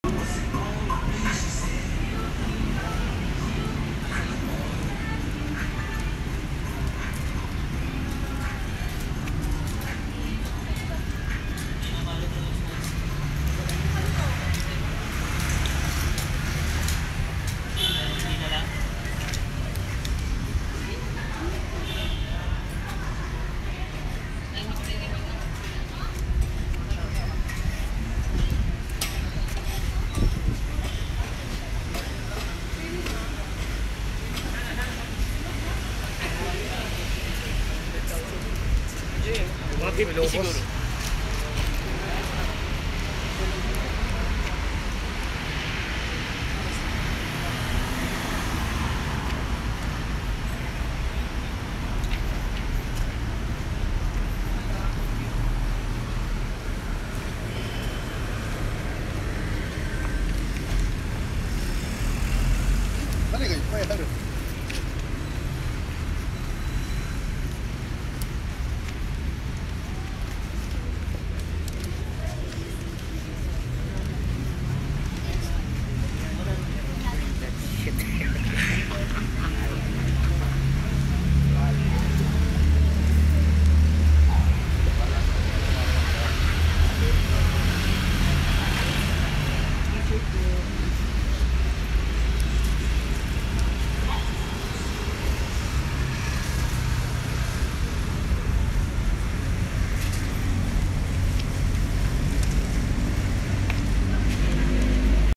Food bazaar in Newport City | Feb. 24, 2024
Quick look at the food bazaar in Newport City, Metro Manila, PH. With added video effect using the Dazz Cam app.